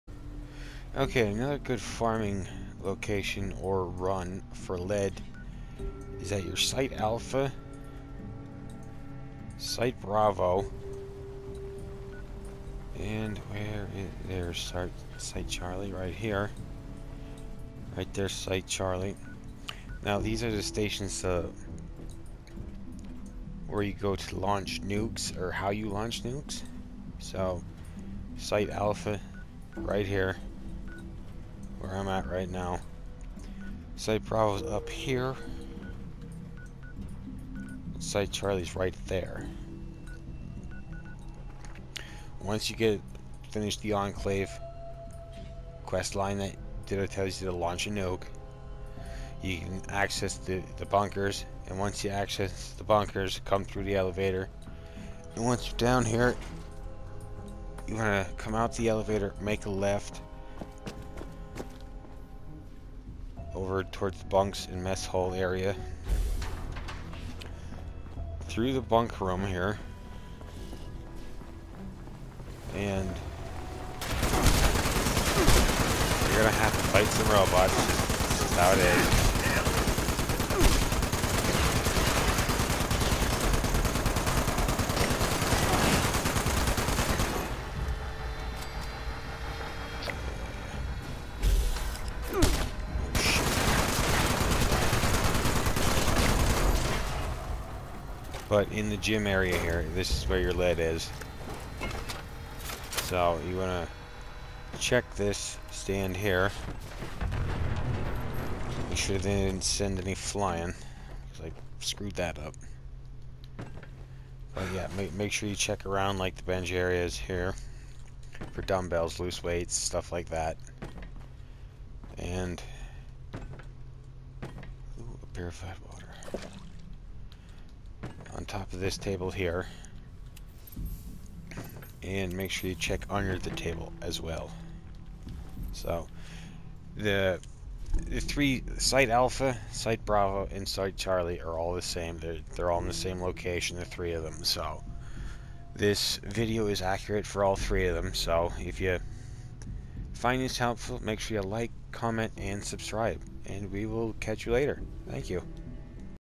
Nuke Silo Lead Farming Locations
This video is accurate for all 3 silos. The lead you're looking for is in the gym area (all the free weights, dumbells, and barbells).